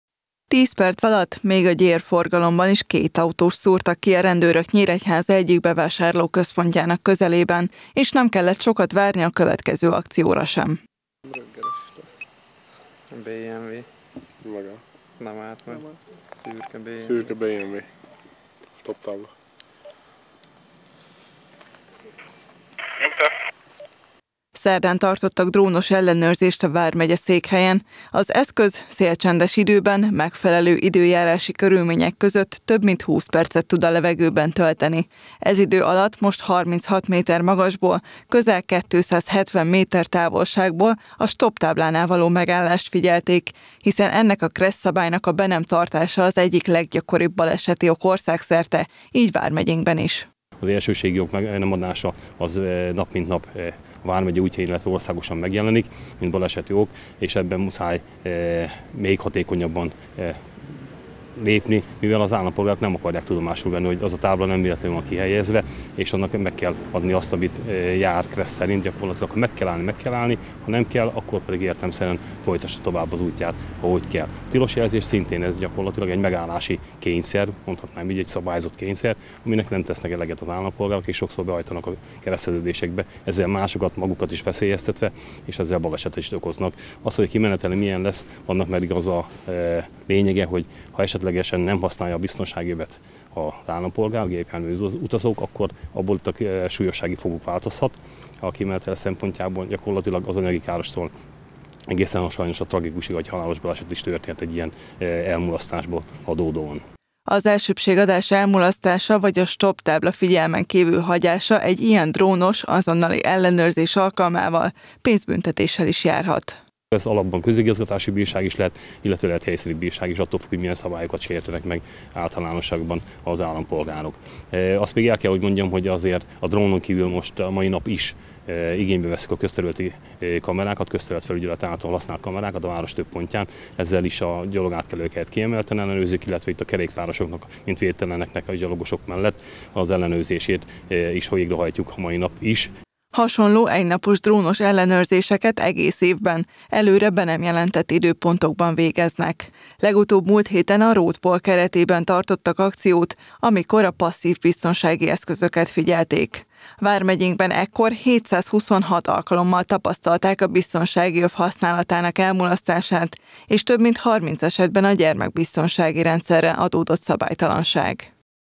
Drónnal figyelnek
Összesen 87 alkalommal intézkedtek a rendőrök a kedden délelőttől délutánig tartó drónos ellenőrzés során Nyíregyházán. A hatóság a Debreceni út és a Tünde utca kereszteződésénél szűrte ki madártávlatból a szabálytalanul közlekedőket. Az akció során főként a biztonsági öv használatának elmulasztása, a STOP tábla és a piros jelzés figyelmen kívül hagyása, valamint sebességtúllépés miatt bírságolt a rendőrség a város egyik legbalesetveszélyesebb pontján.